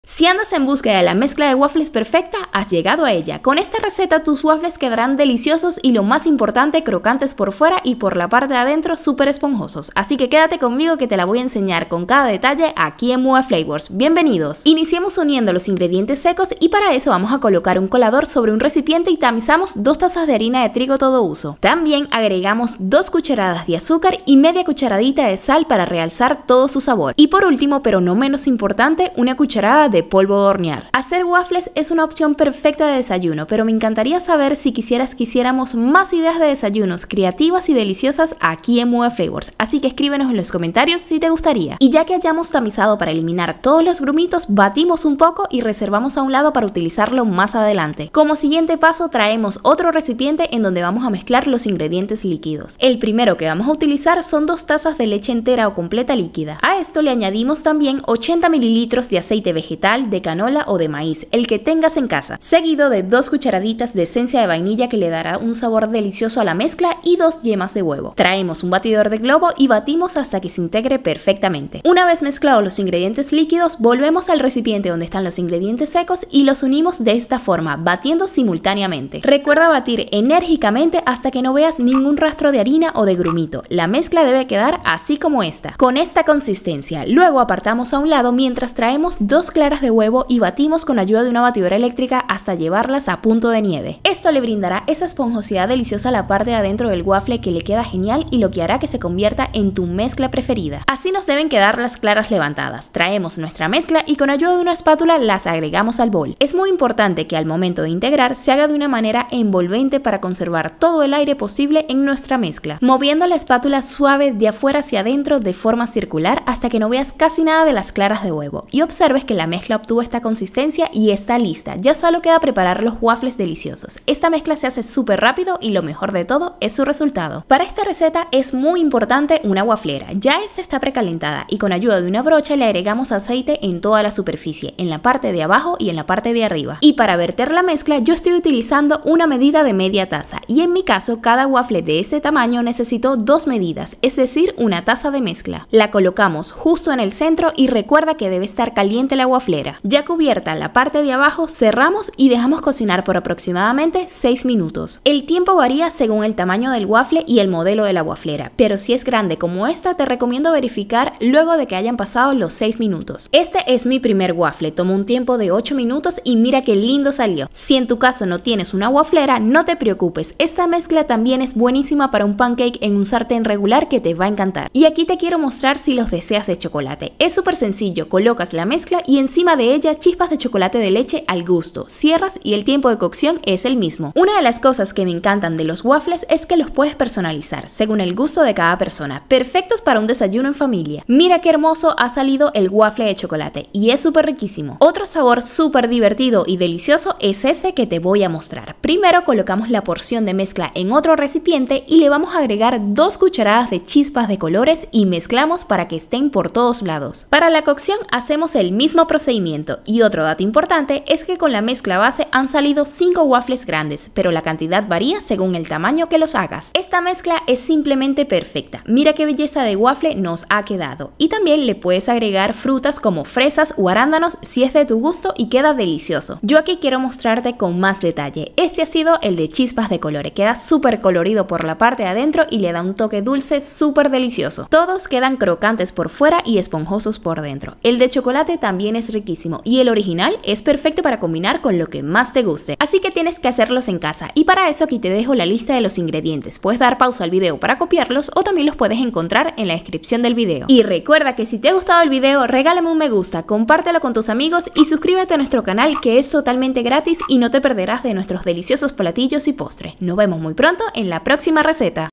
CÓMO HACER WAFFLES 🧇 | ESTA ES LA MEZCLA PERFECTA | Muve Flavors
Has llegado a la receta correcta, esta es la mejor mezcla de waffles que encontrarás y no te imaginas lo fácil y deliciosas que es. Los waffles quedan crocantes por fuera y suavecitos y espojositos por la parte de adentro. Además te enseño cómo hacerla con ingredientes fáciles de conseguir y además las puedes personalizar según el gusto de cada persona, es perfecta para compartir en familia. Y si no tienes waflera no te preocupes, con esta mezcla también quedan unos pancakes súper deliciosos.

Waffles perfectos
Ingredientes:
2 tazas de harina de trigo todo uso (300 gr)
1 Cda de polvo de hornear
1/2 Cdita de Sal
2 Cdas de azúcar
2 Huevos
80 ml de aceite (5 Cdas)
2 Cditas de esencia de vainilla
2 tazas de leche líquida (500 ml)

Waffles con chocolate:
Agregar chispas de chocolate de leche (2 Cdas por waffles)

Waffles con chispas de colores:
Integrar  2 Cdas de chispas de colores a cada porción de la mezcla de waffles.

Cantidad de waffles: 5 grandes. (1 taza de mezcla para cada uno)
Nota: la cantidad de waffles varía según el tamaño de la waflera que utilices.